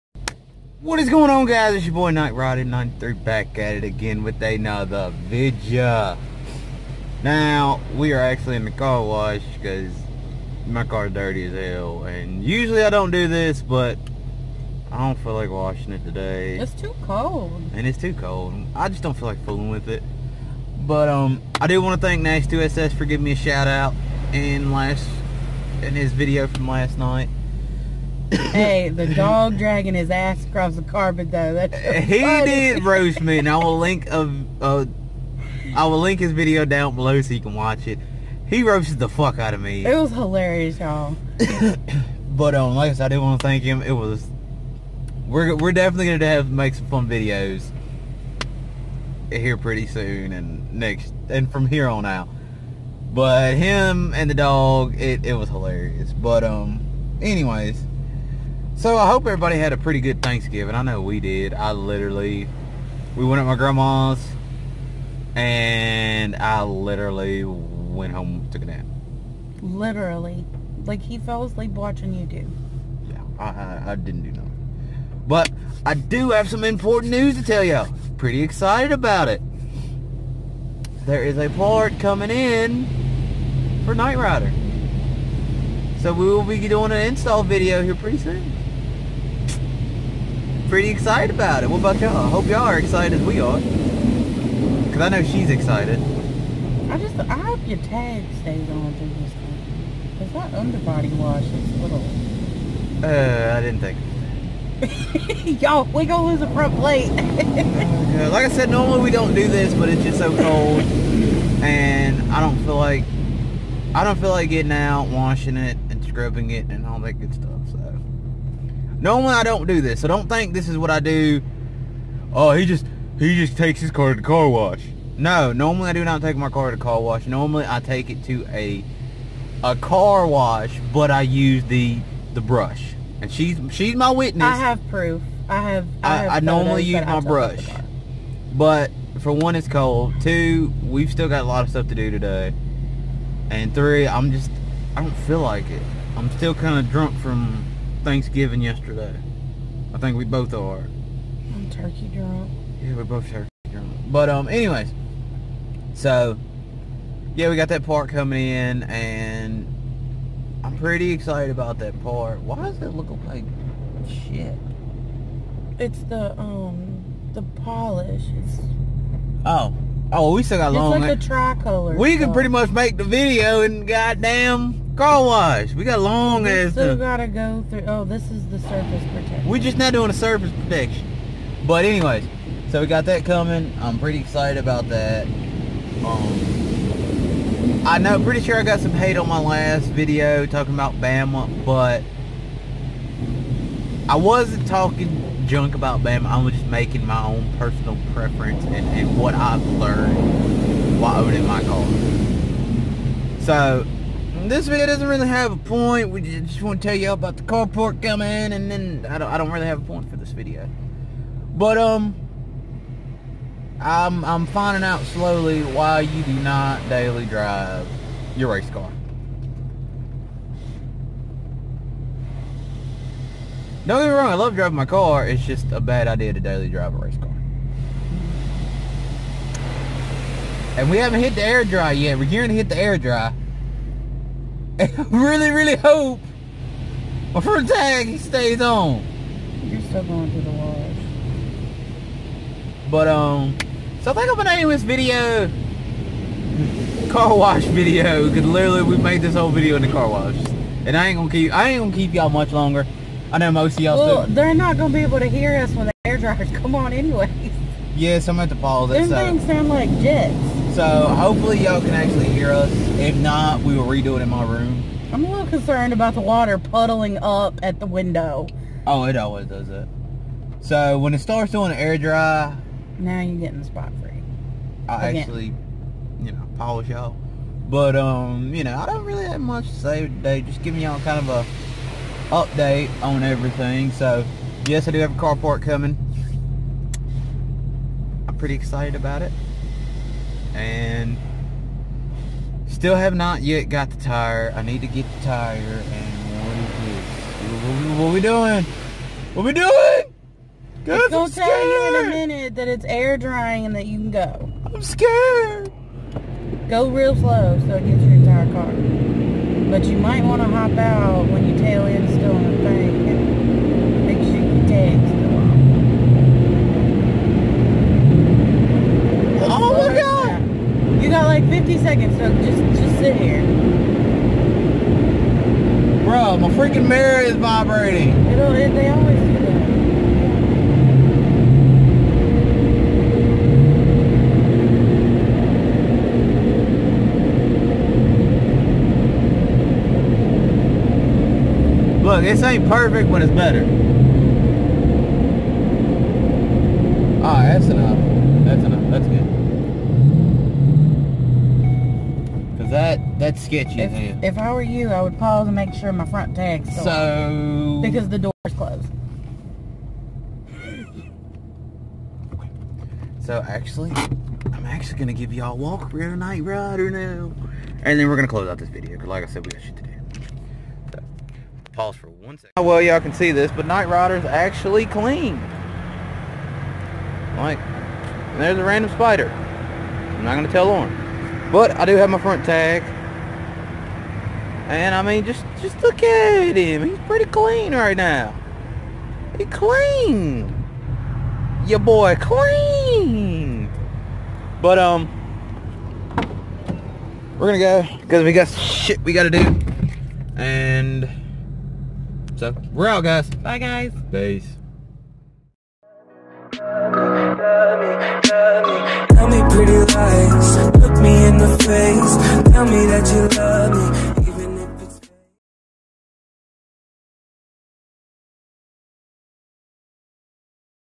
Car Wash Vlog
Today,s video we talk about the new mod i have comin for my car. Plus I give a response to Nasty2ss video off me calling him out. Please watch the video it is hilarious. Not much going on just a car wash and response.